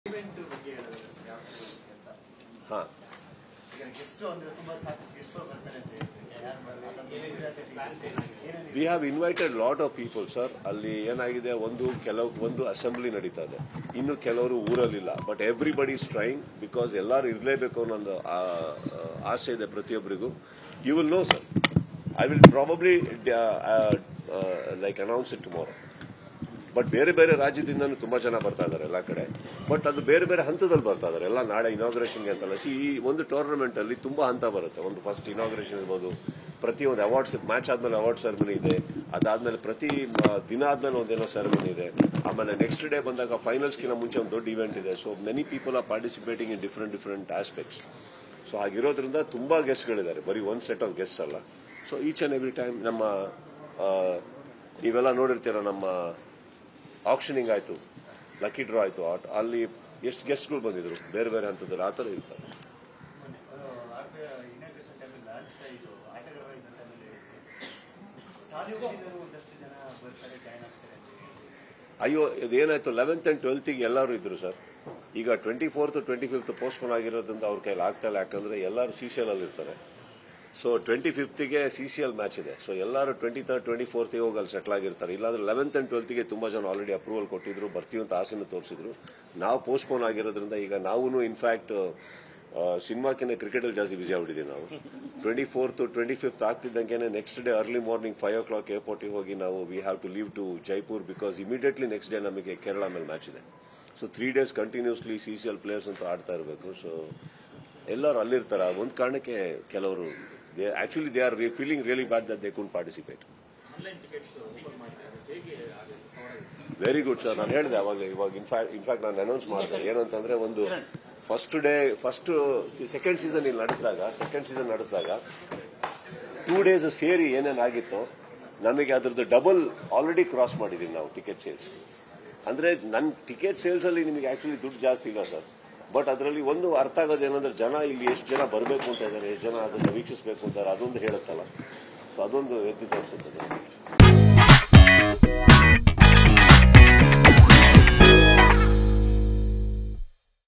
Kichcha Sudeep | ಬೇರೆ ಬೇರೆ ರಾಜ್ಯದಿಂದ್ಲೂ ಸ್ಟಾರ್ಸ್ ವಿಸಿಟ್!| KCL | Vistara Cinema
KCCಗೆ ಭರ್ಜರಿಯಾಗಿ ಸಿದ್ಧತೆ ನಡೆದಿದ್ದು, ಈ ಬಗ್ಗೆ ನಟ ಸುದೀಪ್ ಪ್ರೆಸ್​ಮೀಟ್ ನಡೆಸಿ ವಿವರಿಸಿದ್ದಾರೆ.